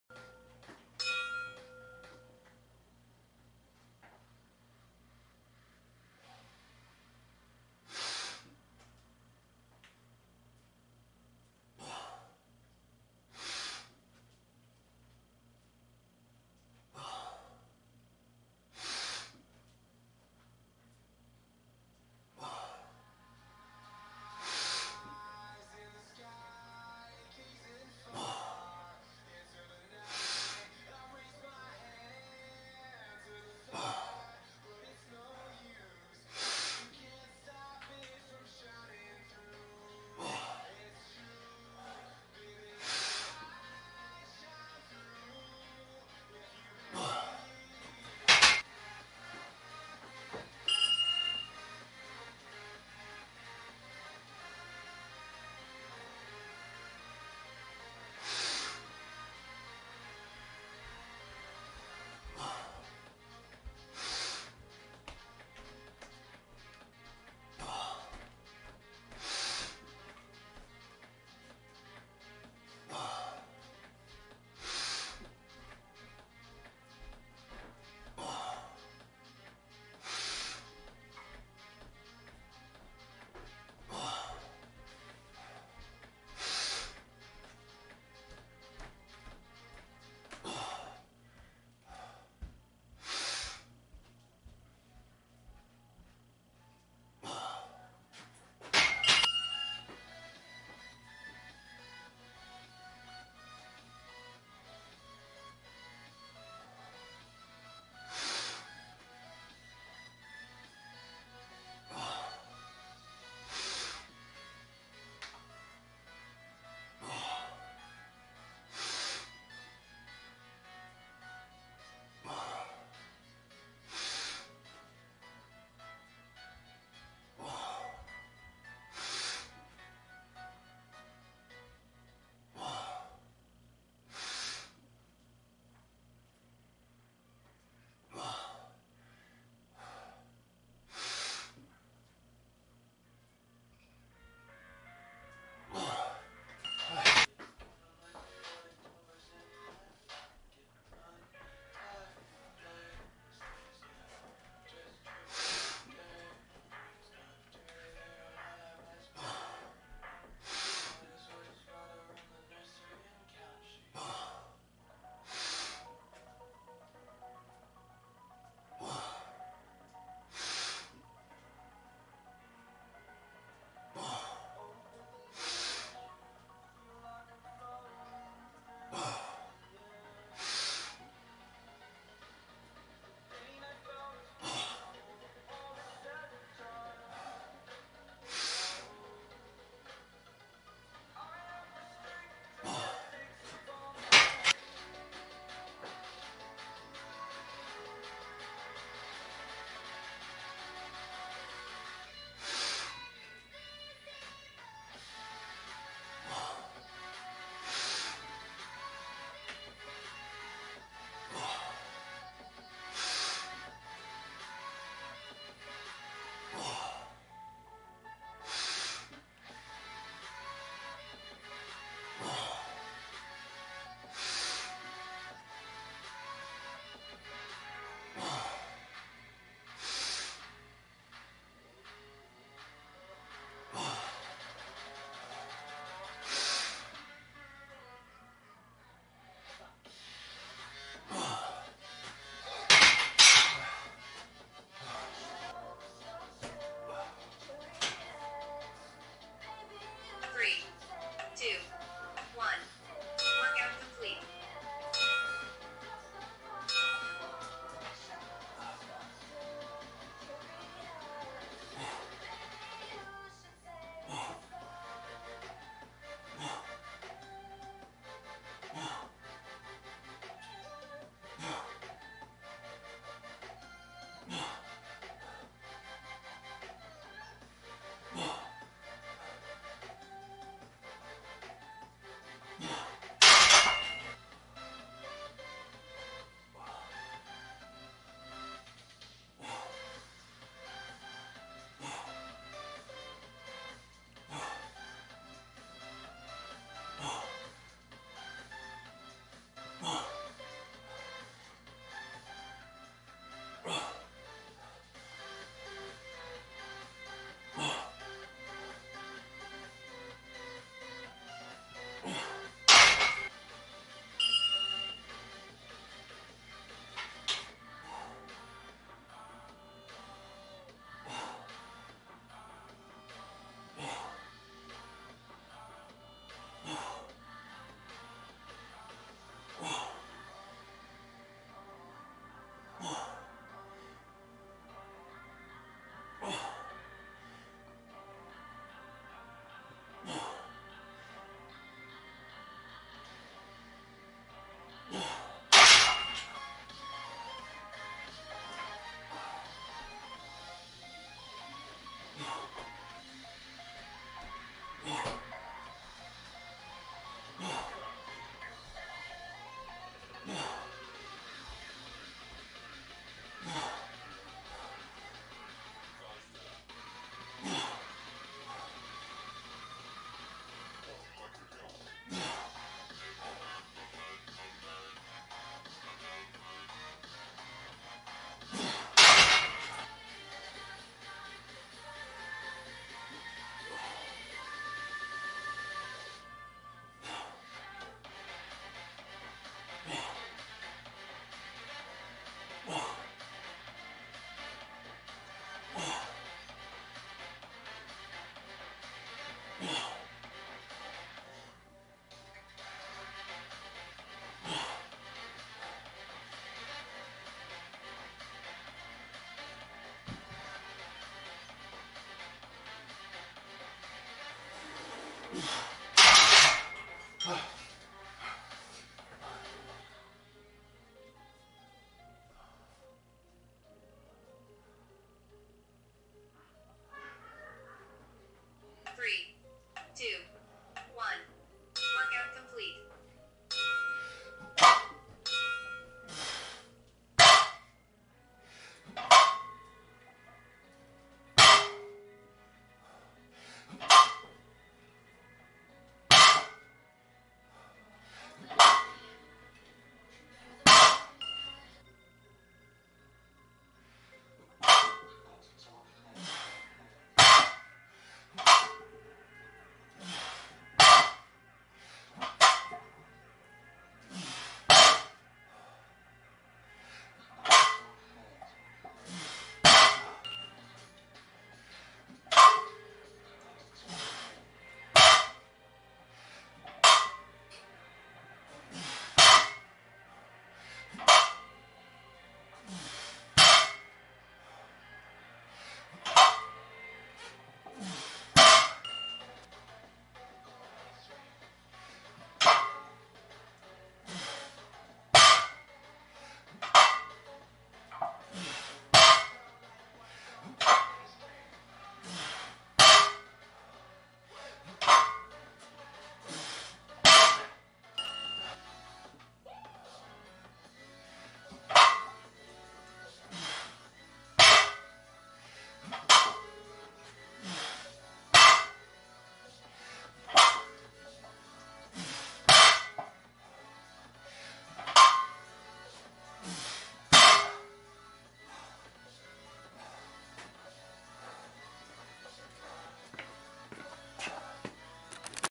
StrongLifts 5x5* Day 365 Workout A; **PR: Rows 5x4 @ 220lbs**
Today:
Squats*: 5x7 @ 120lbs
Bench Press: 4x8,7 @ 165lbs
Rows: 5x4 @ 220lbs (PR)
Close Grip: Skipped @ 155lbs
Recovery Times:
Squats 3:00, Bench 3:00, Rows 3:00, Close Grip 2:00, (Fails 5:00)
*Reset for hips, knees and ankles; 5lb plates under heels

My current plan is to Squat with Reps ranges of 5x8-10 Monday, 5x6-8 Wednesday, and 5x5 Friday.
Also, Bench and OHP will only increase once I can do 5x8.

Let's GO!!